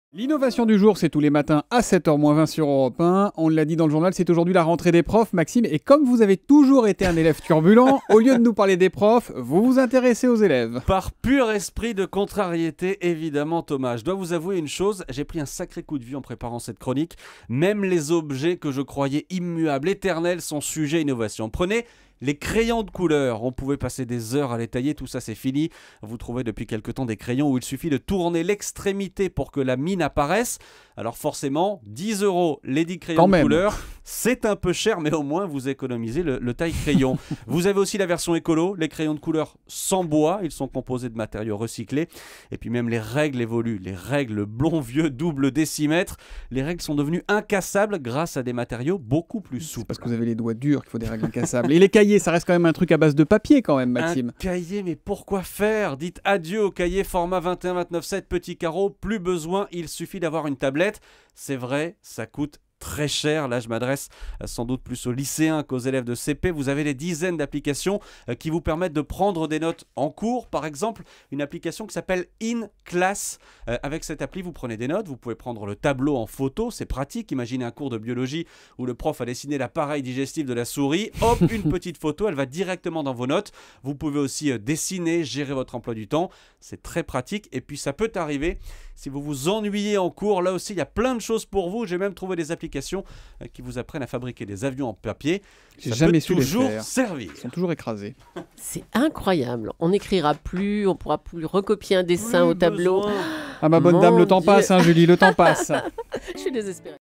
La rentrée nouvelle génération
Chaque objet de la rentrée scolaire a eu le droit à bon nombre de changements et d'innovations.